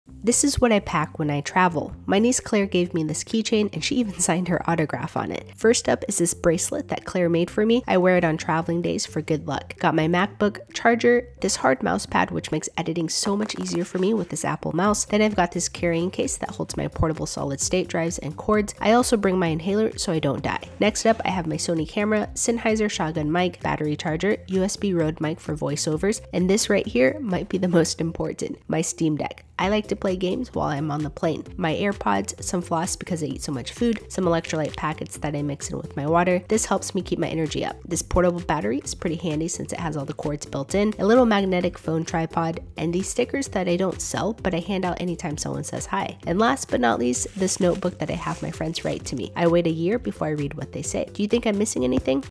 What I Pack When I Travel
Travel Backpack
Electronics Organizer
Camera Body
Camera Lens
Shotgun Mic
Battery Charger Case
Voiceover Mic
Electrolyte Packets
Phone Tripod